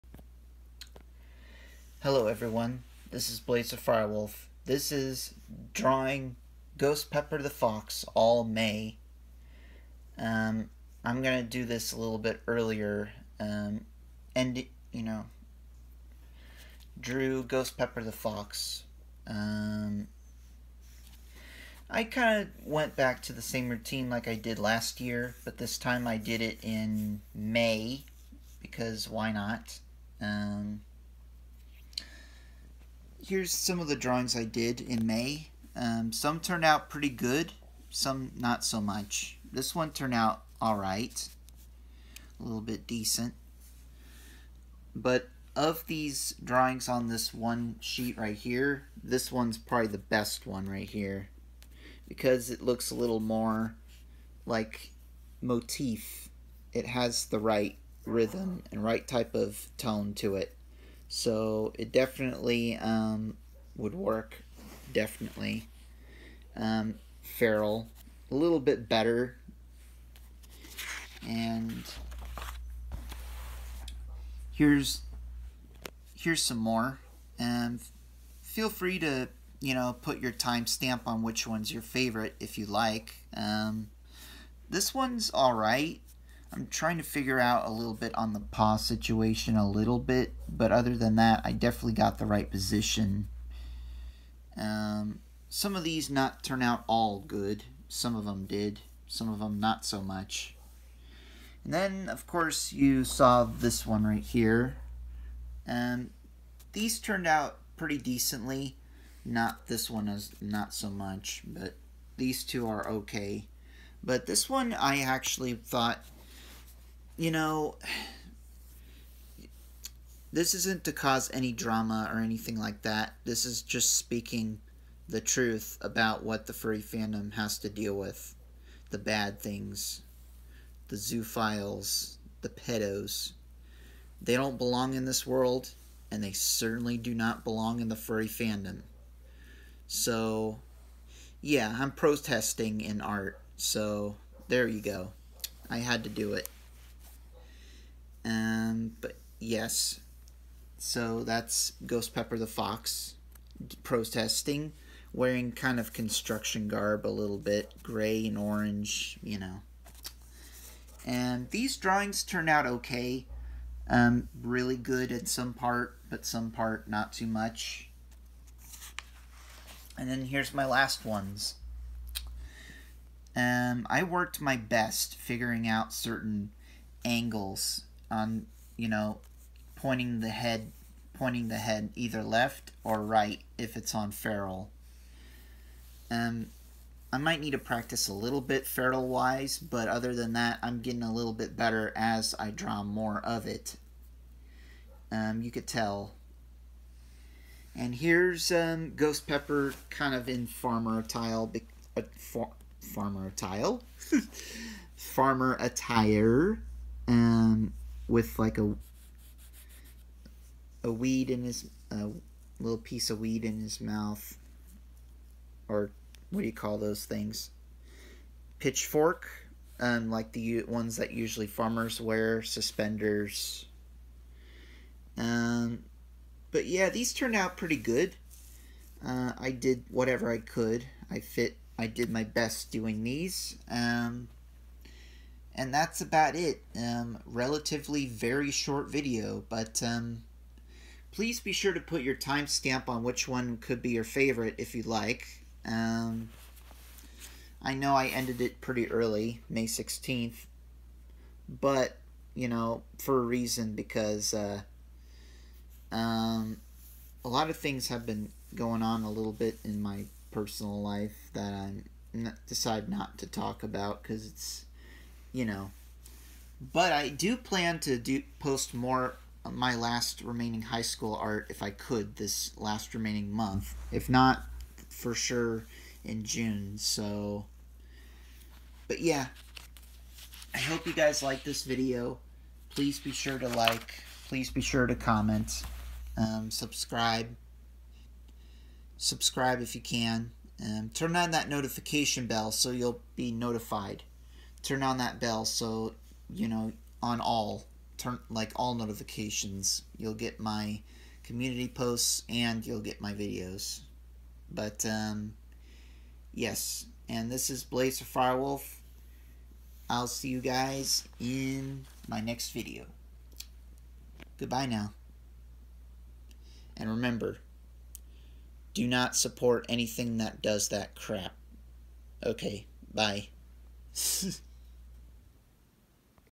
Drawing Ghost Pepper The Fox All May
Here’s drawings I did of Ghost Pepper The Fox for May I hope you enjoy these 💕💕